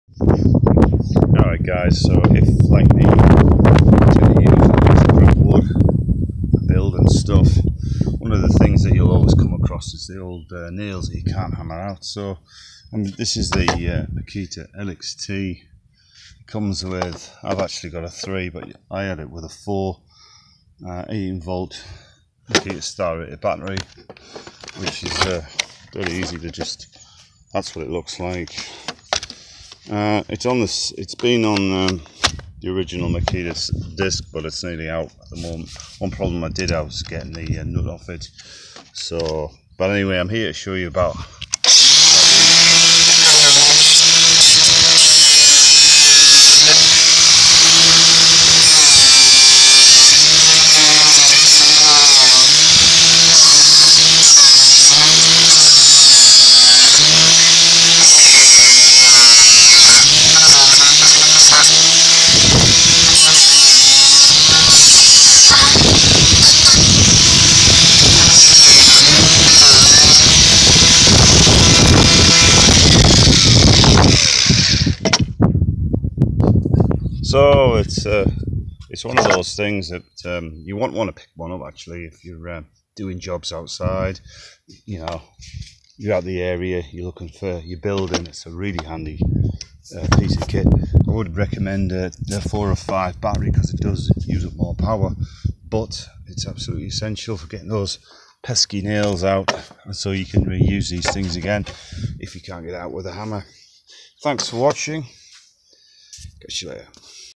Busting rusty old nails is easy with makita cordless grinder (DAA452 115mm )
good machine really handy if your in the field without power😎👍